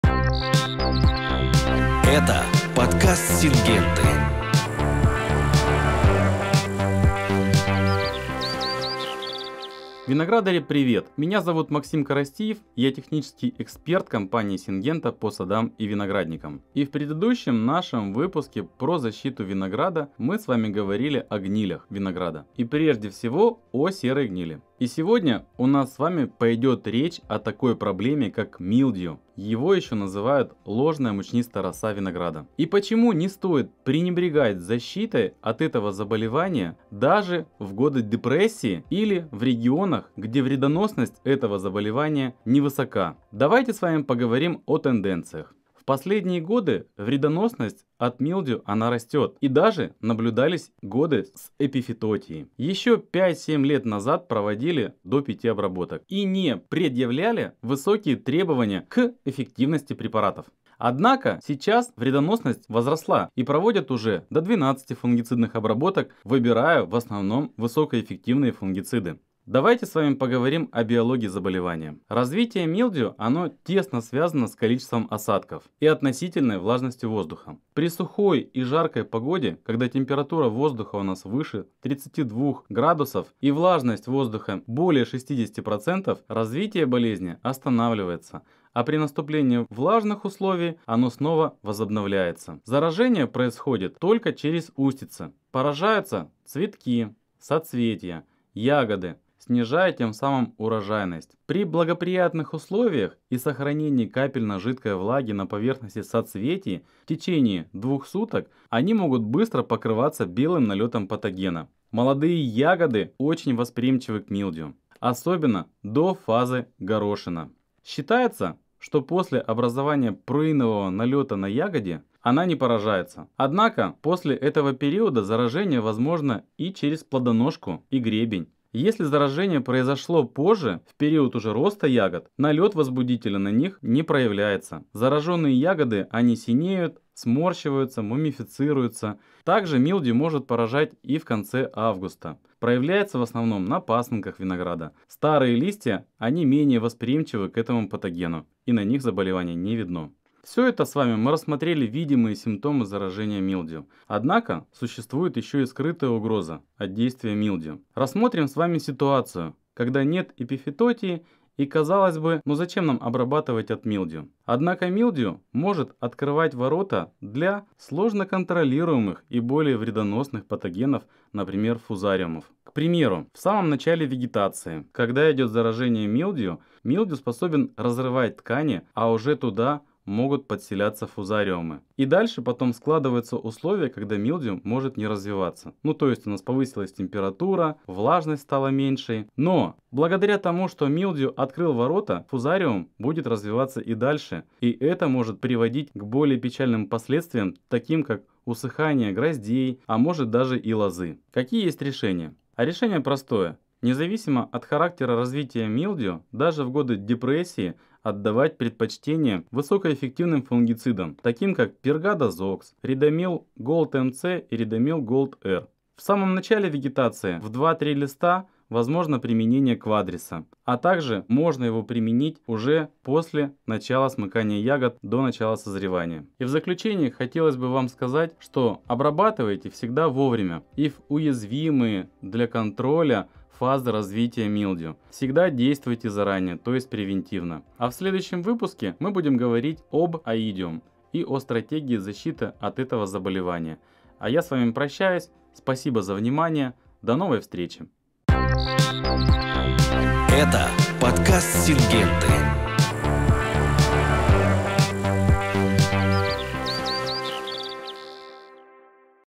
🍇 Милдью винограда: явные и скрытые угрозы вредоносности
В новом выпуске подкаста «Сингенты» технический эксперт компании Максим Коростиев делится о явных и скрытых угрозах вредоносности милдью винограда.

Появились вопросы к специалистам? Напишите нам в Телеграм или ВКонтакте!